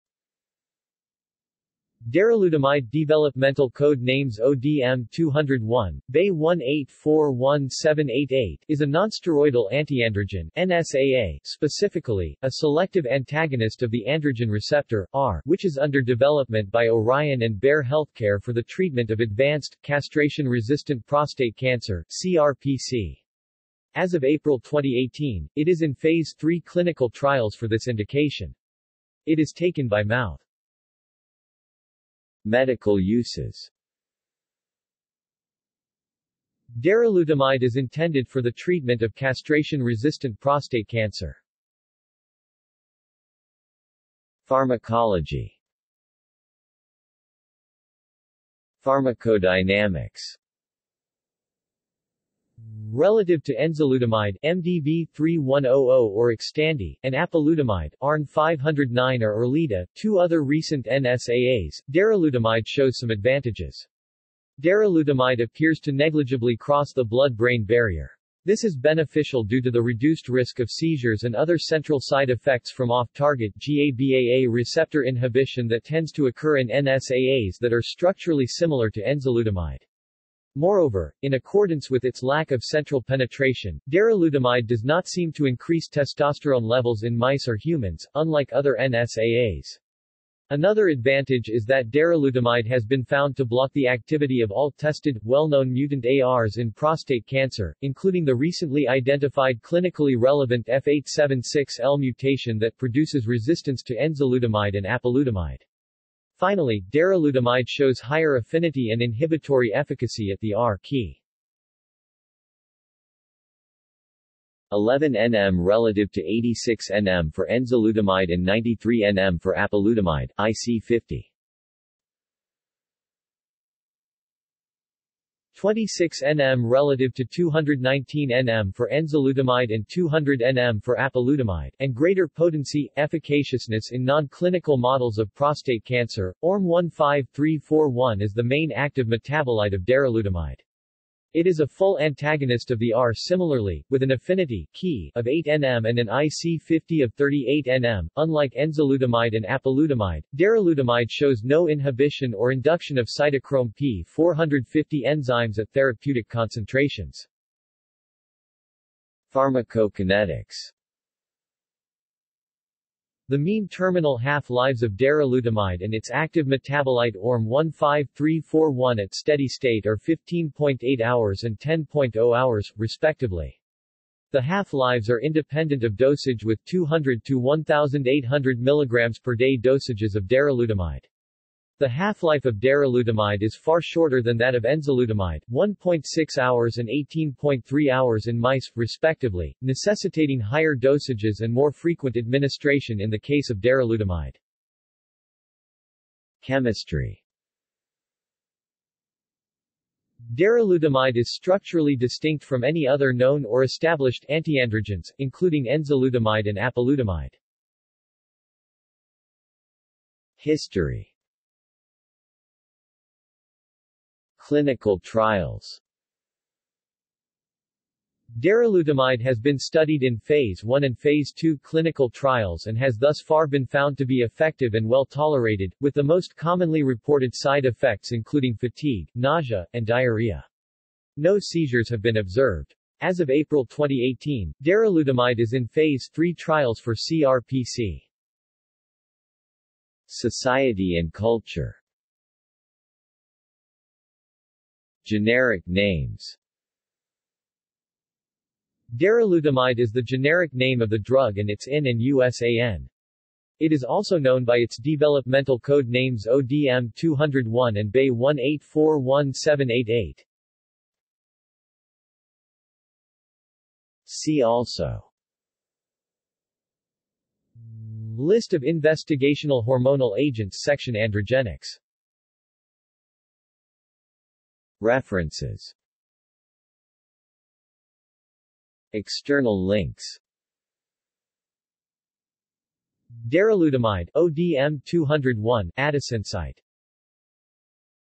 Darolutamide | Wikipedia audio article
This is an audio version of the Wikipedia Article:
Darolutamide

SUMMARY
Darolutamide (developmental code names ODM-201, BAY-1841788) is a nonsteroidal antiandrogen (NSAA) – specifically, a selective antagonist of the androgen receptor (AR) – which is under development by Orion and Bayer HealthCare for the treatment of advanced, castration-resistant prostate cancer (CRPC). As of April 2018, it is in phase III clinical trials for this indication. It is taken by mouth.